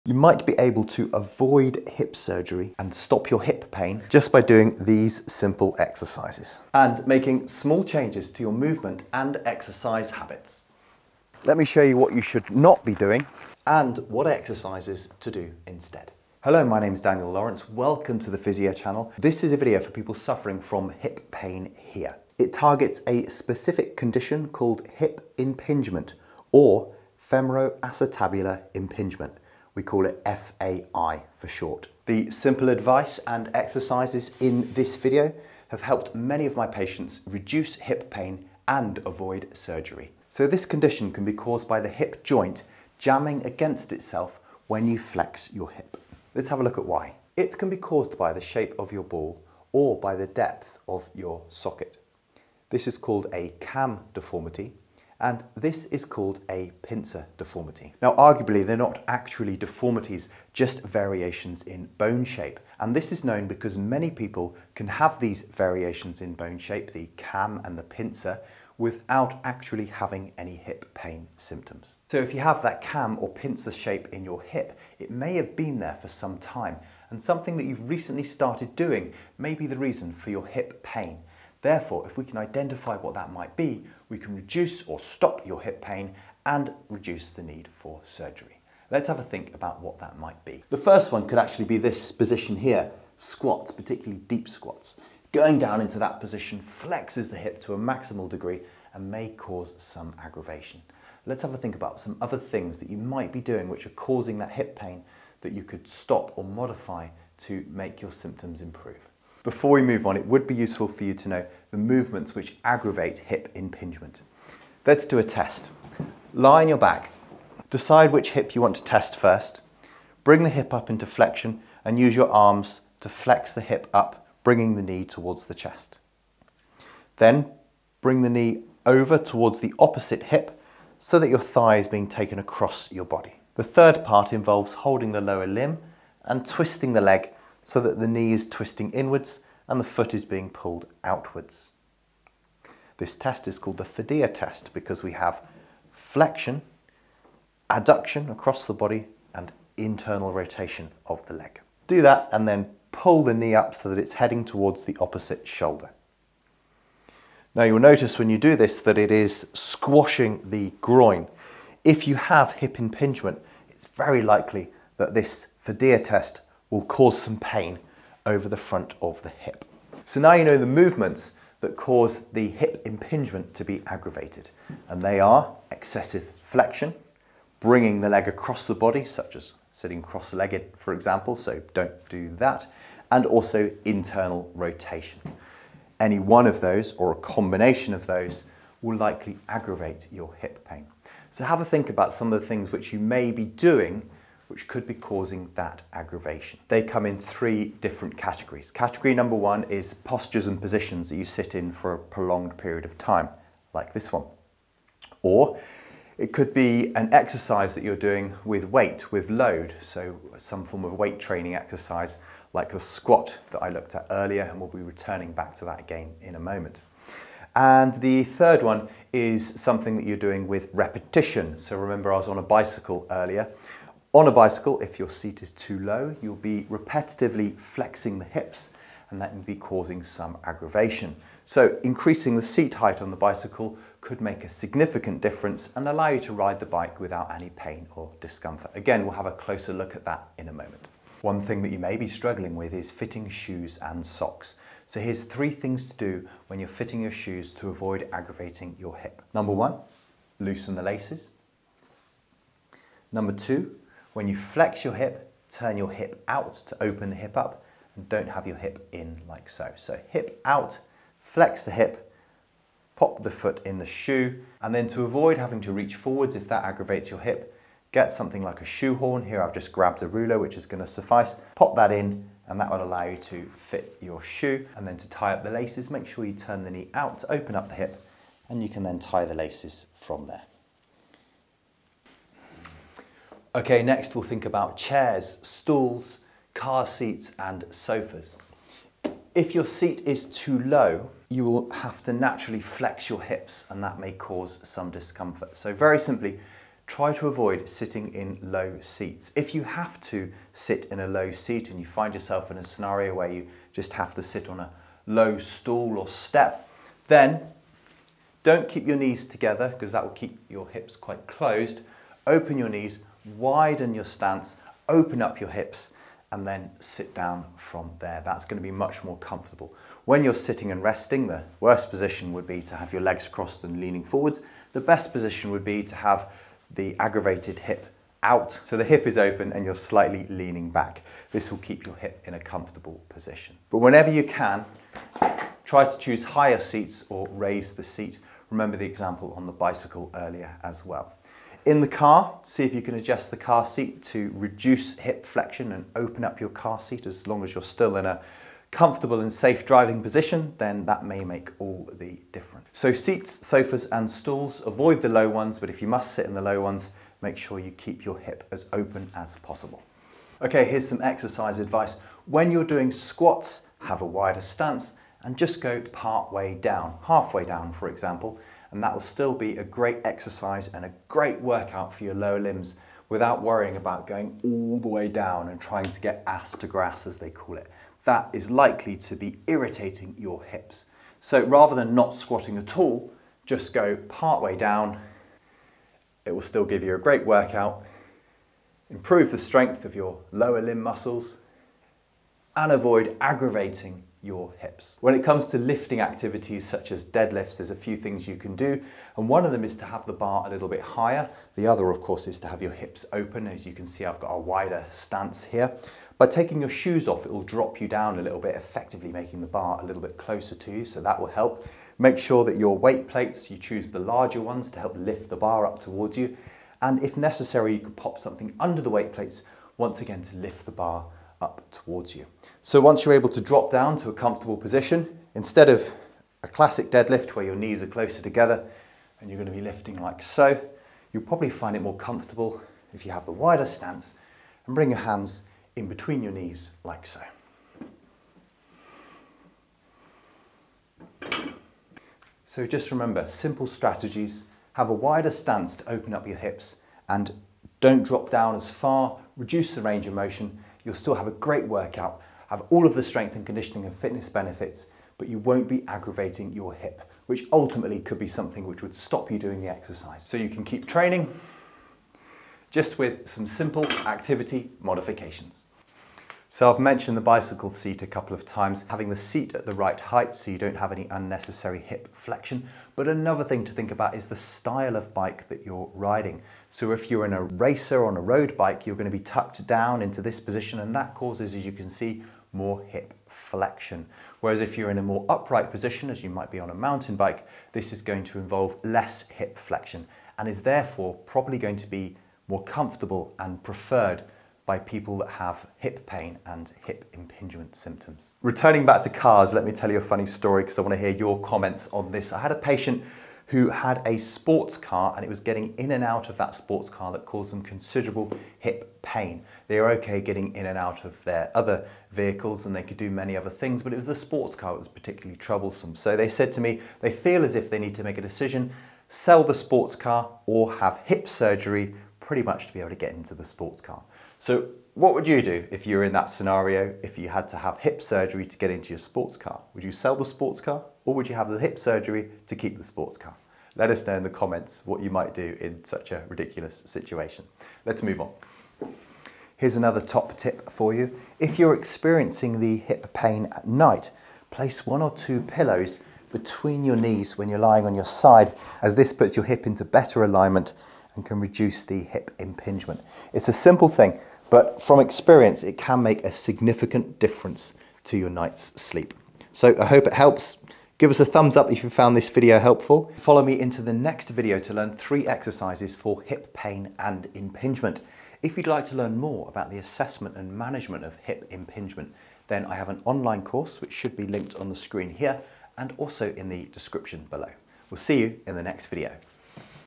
STOP HIP PAIN QUICKLY | Femoroacetabular impingement | FAI
In this video Physical Therapist & Health Professional Daniel Lawrence explains femoroacetabular impingement of the hip also known as FAI. Daniel demonstrates ways to relieve and reduce hip pain quickly and naturally so you can avoid surgery and medications whenever possible. FAI often causes labral tears, so this self help video will also help with this type of injury.